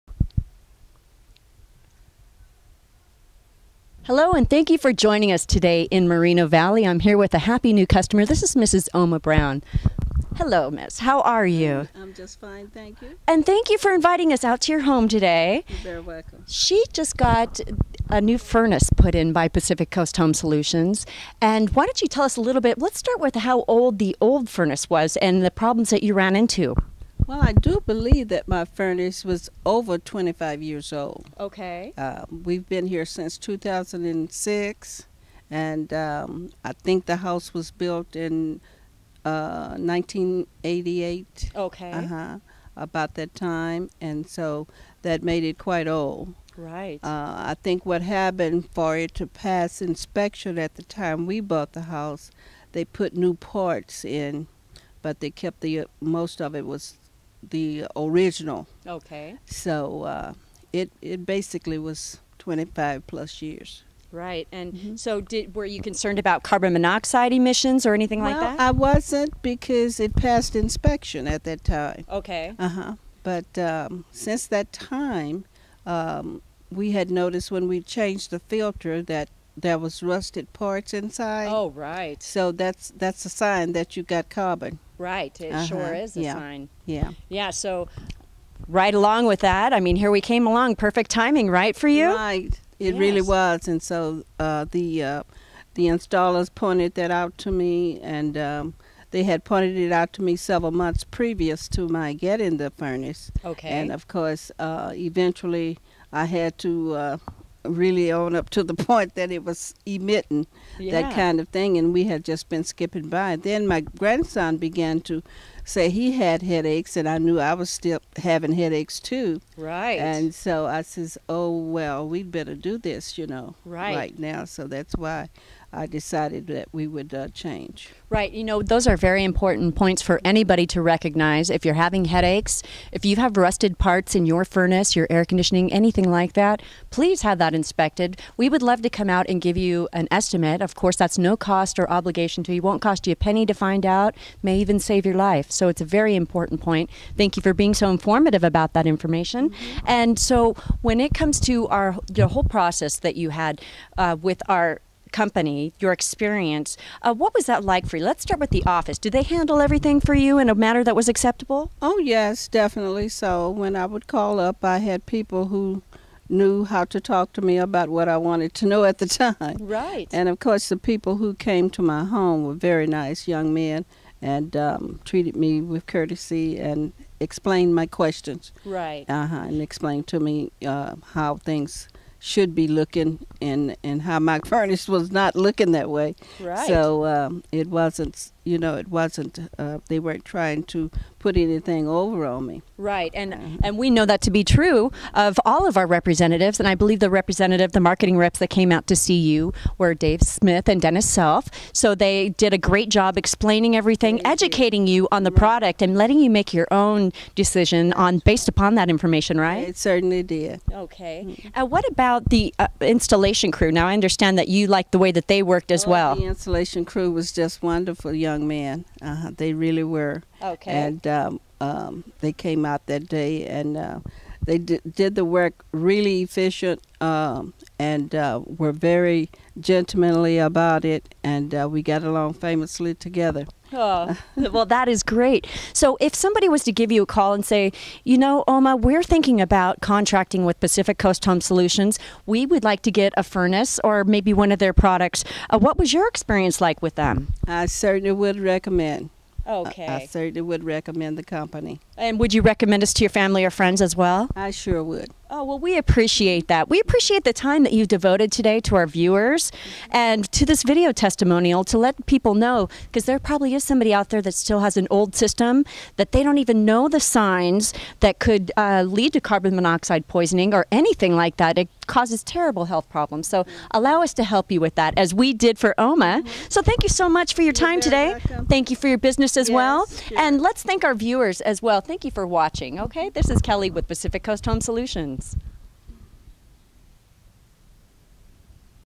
Pacific Coast Home Solutions - (HVAC) Oma B. Testimonial Moreno Valley, CA
Kellie Chaney with Pacific Coast Home Solutions shows us the beautiful job PCHS did at Oma's home in Moreno Valley while installing her new HVAC unit.